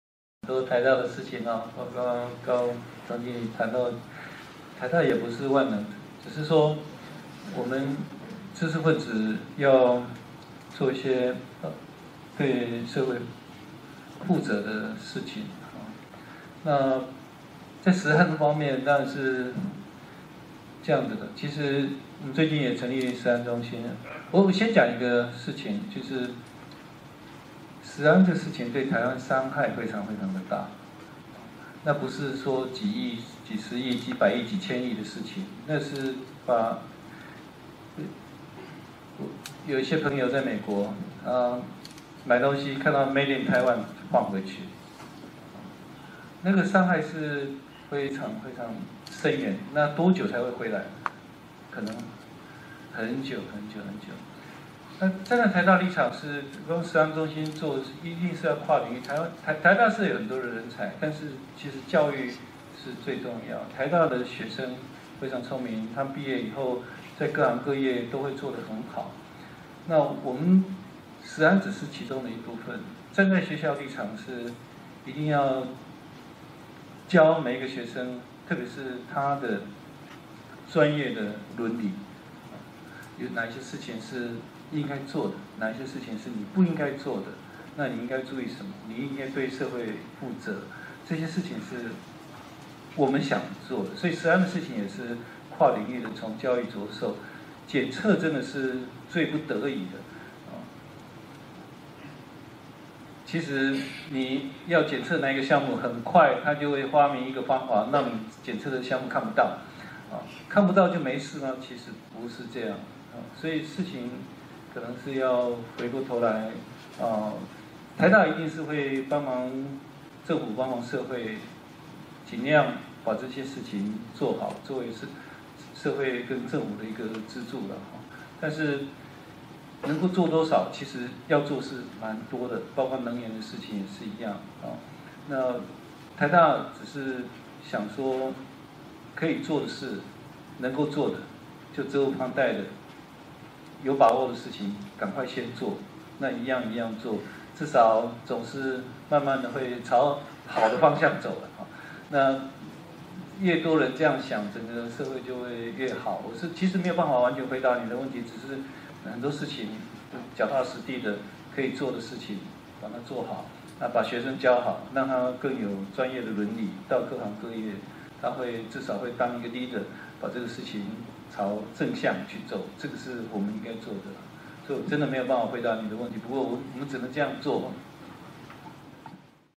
鉅變新視界沙龍：台灣未來社會褐色何去，綠色何從？－知識分子責任
由臺灣大學社會科學院風險社會與政策研究中心主辦，2015年1月7日（三）於臺灣大學社會科學院一樓108室舉行，沙龍主題：台灣未來社會褐色何去，綠色何從？
本段影片講者（按影片講話順序排序）：
楊泮池校長（臺灣大學校長）